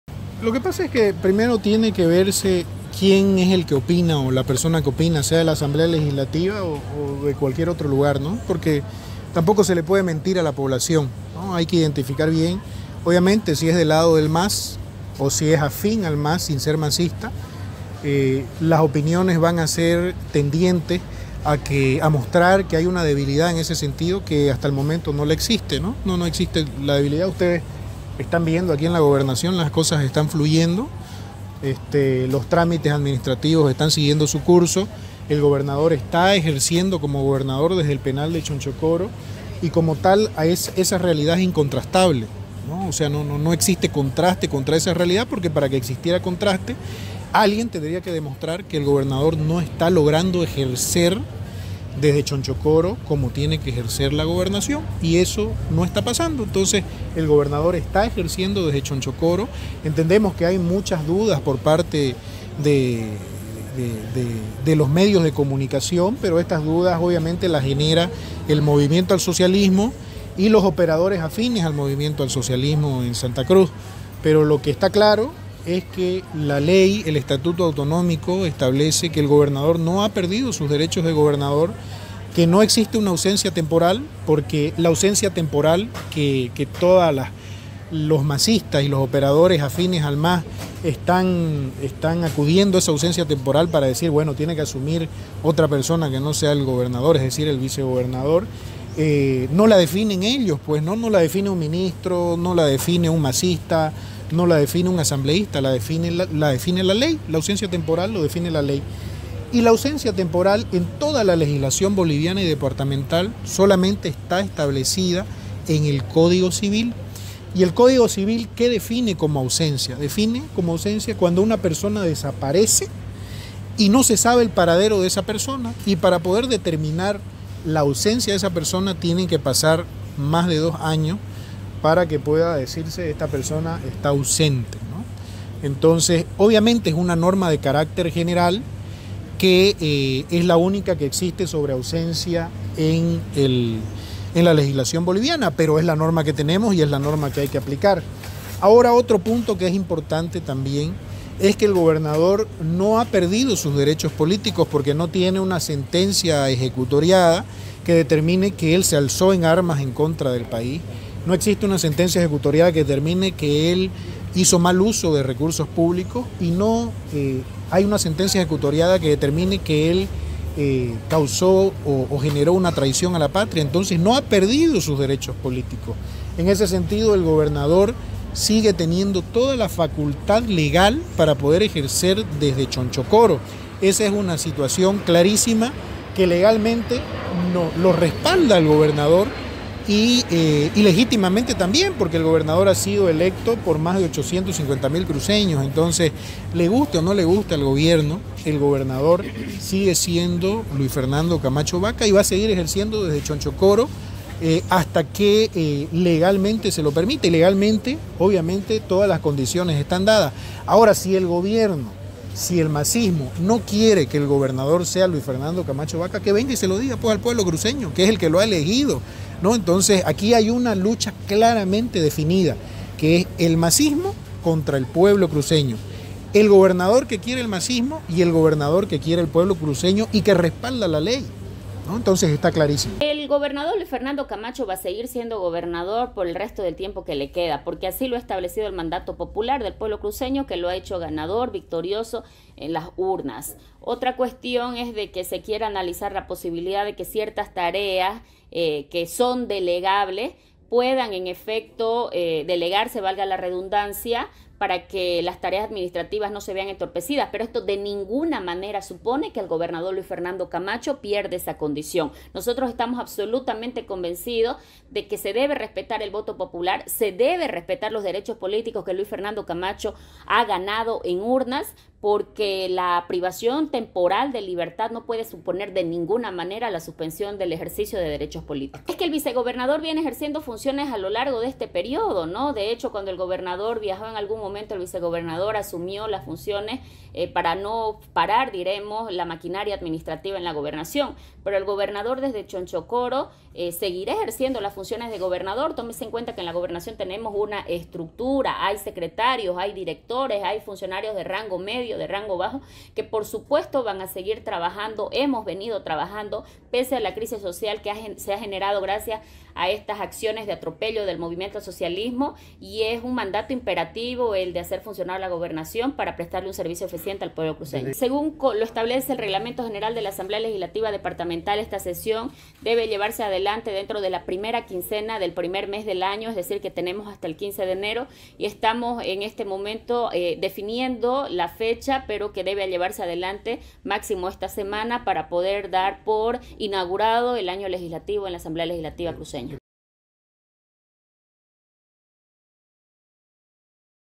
La Gobernación cruceña sigue funcionando y Camacho seguirá ejerciendo desde Chonchocoro, descartan c
La Gobernación cruceña sigue funcionando y Camacho seguirá ejerciendo desde Chonchocoro, descartan cambio porque no hay vacio